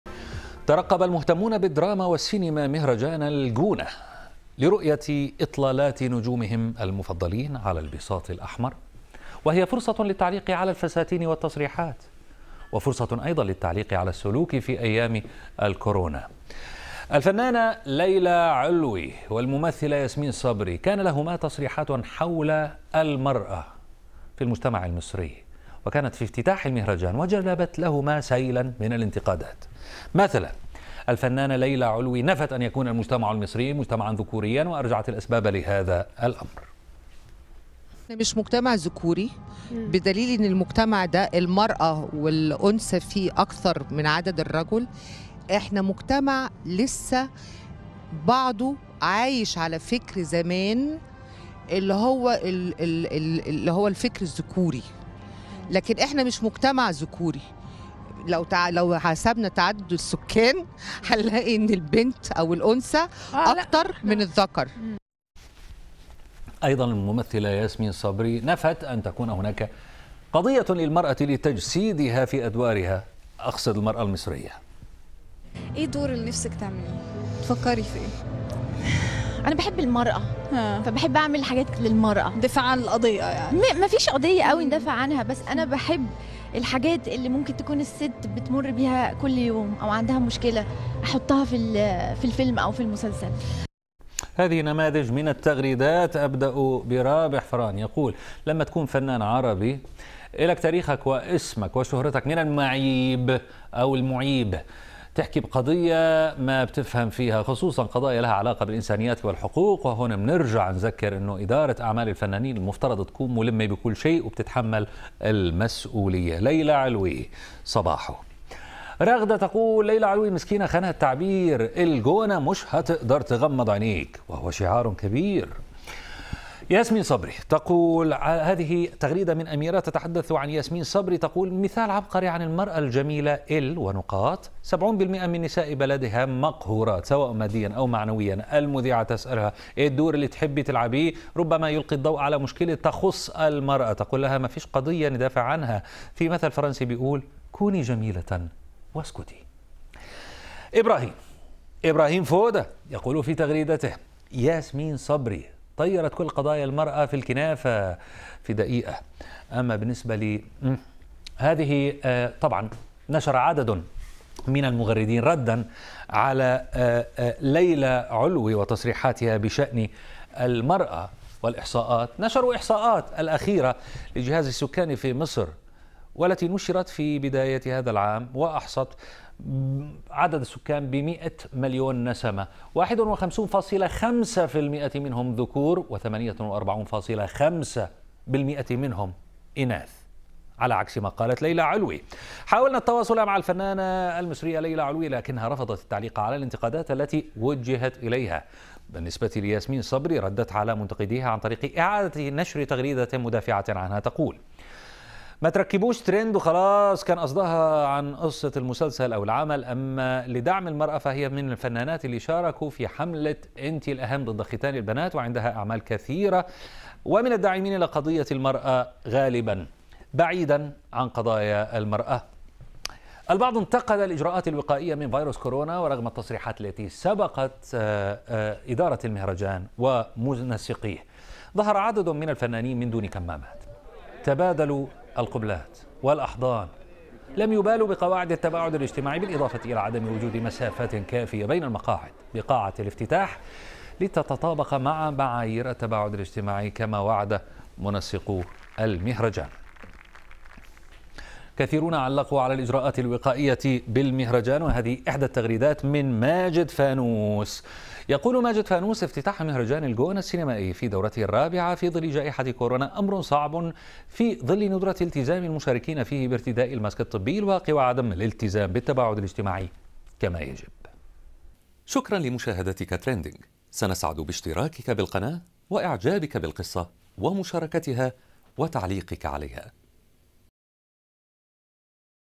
انتقادات لمهرجان الجونة وتفاعل مع تصريحات ليلى علوي وياسمين صبري
مع انطلاق الدورة الرابعة لمهرجان الجونة السينمائي بدأت الانتقادات حول عدم اتباع التباعد الاجتماعي وبسبب تصريحات جدلية لبعض الفنانات.